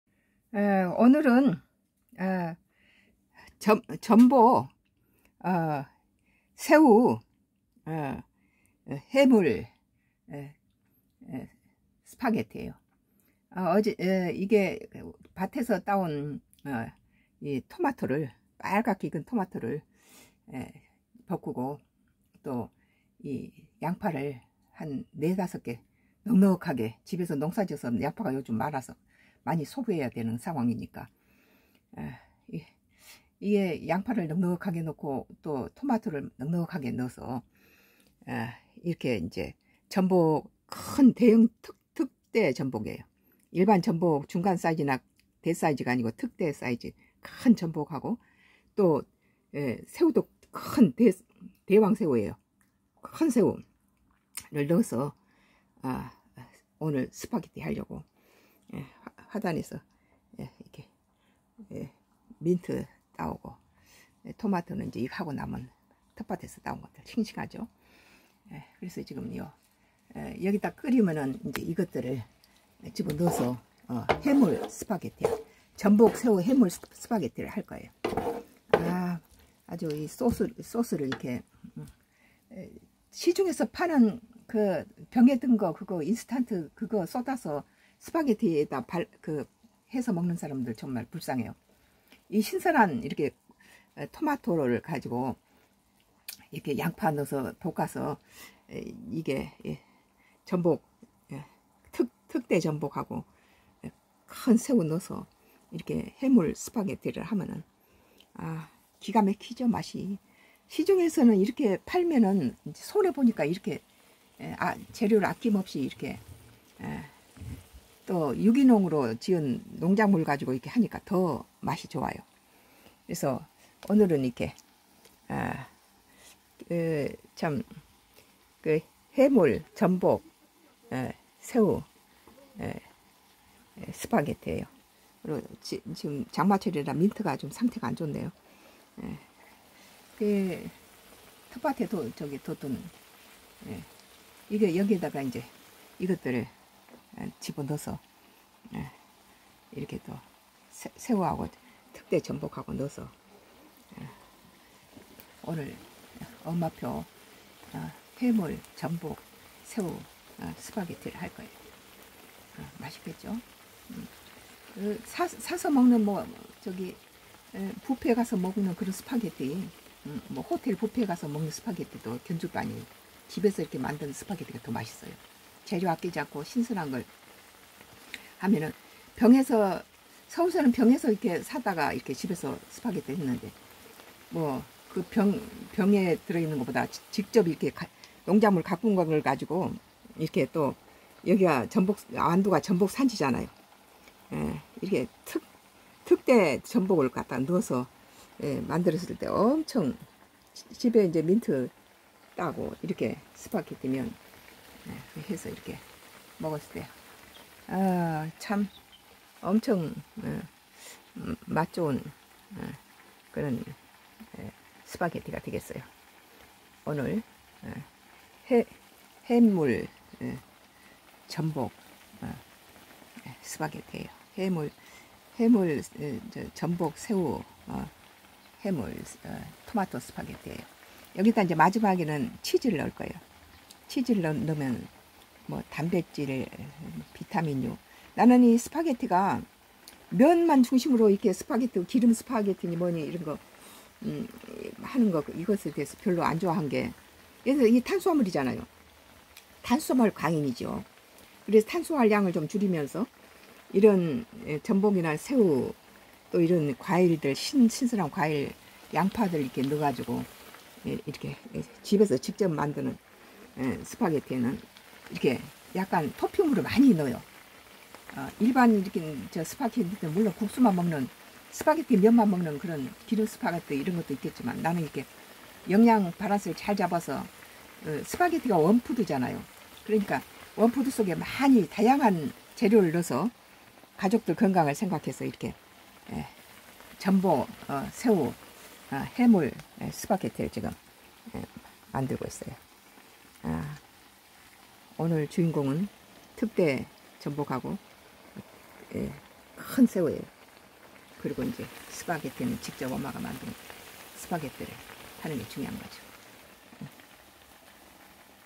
MAMA FOOD엄마표! 전복 새우 토마토 해물스파게티~~~~~!@sungodcross
엄마표! 전복 새우 토마토 해물스파게티 !

스파게티를 좋아하는 우리가족은~~~양파와 토마토가 풍성한 이 계절엔 ! 여러가지 종류의 스파게티를 자주해 먹는다~~~!

 오늘은! 전복,새우,양파,새우,를 넣은 해물스파게티이다~~~! 맛도 일품이고~~~ 영양도 만점인 해물스파게티는~~! 시중 파는 스파게티와는 맛이나 영양에 견줄바가 아니다~~~!@sungodcross